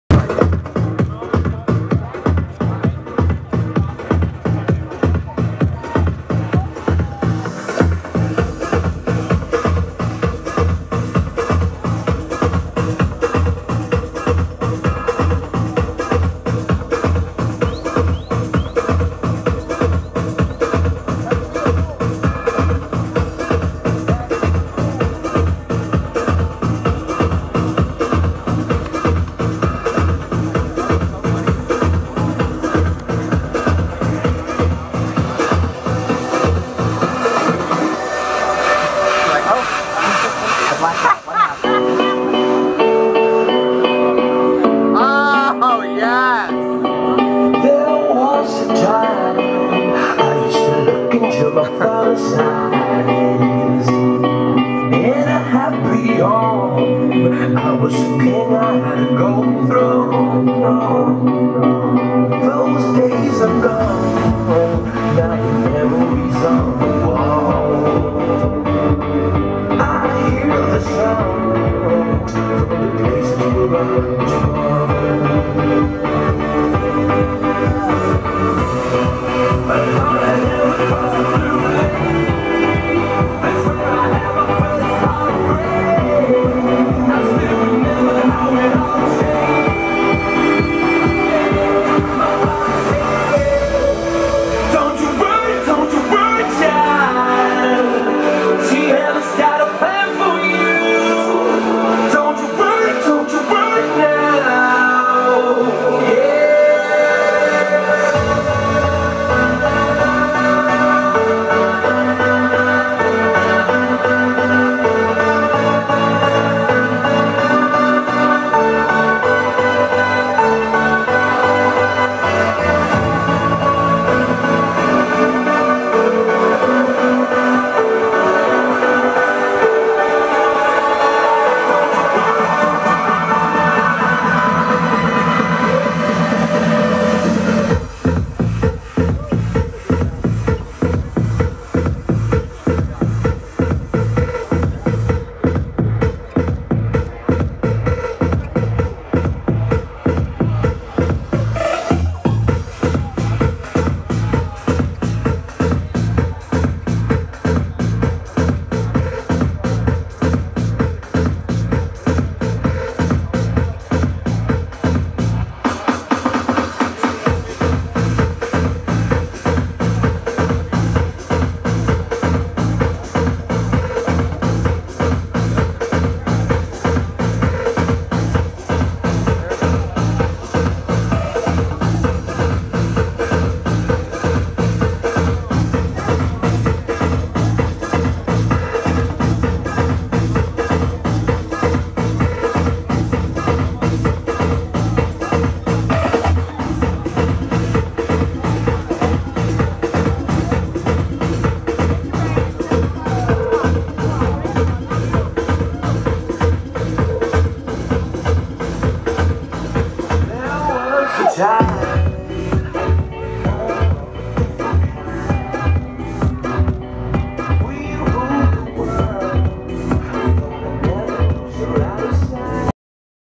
Nina Flowers Spinning in Cleveland 1
Video taken durning the "Rip The Runway" Party at Mean Bull Cleveland featuring DJ Nina Flowers of Denver's Tracks Niteclub.